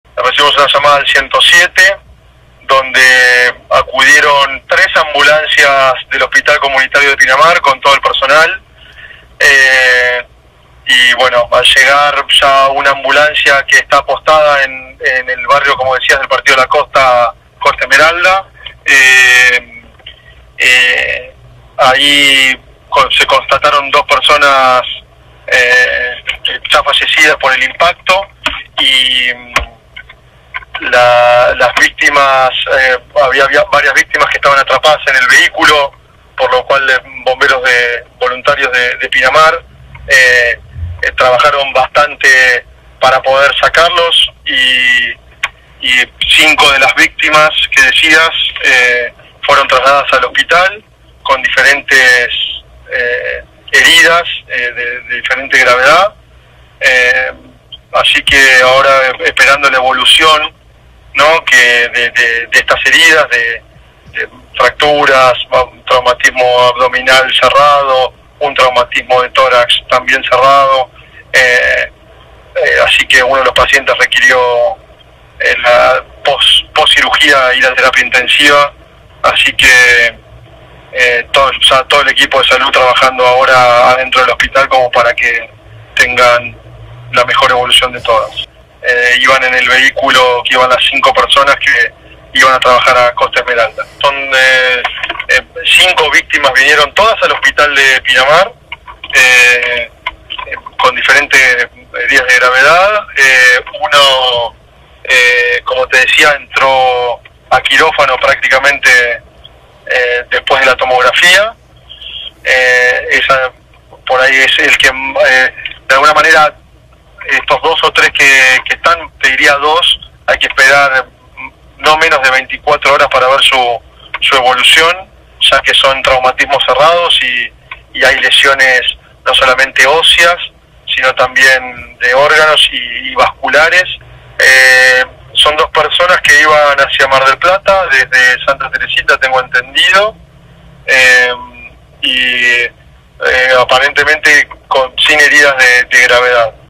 EDUARDO DAGOSTINO FATAL ACCIDENTE EN RUTA 11 KM 385
DOS PERSONAS MURIERON TRAS UN CHOQUE FRONTAL ENTRE DOS AUTOS
ACCIDENTE FATAL EN LA RUTA 11 KM 385

EDUARDO D´AGOSTINO - SECRETARIO DE SALUD PINAMAR

HORACIO DE LABURU - OFICIAL AUXILIAR DE ESCUADRA